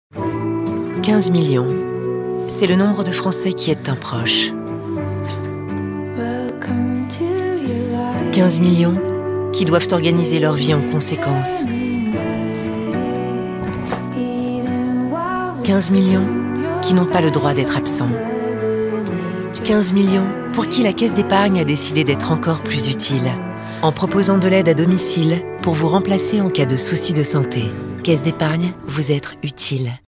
Film TV Aidants – 30 sec – ST
Aujourd’hui, 15 millions de Français aident un proche. Pour être utile aux aidants, la Caisse d’Epargne propose une assurance santé avec de l’aide à domicile pour les remplacer auprès de la personne aidée en cas de souci de santé.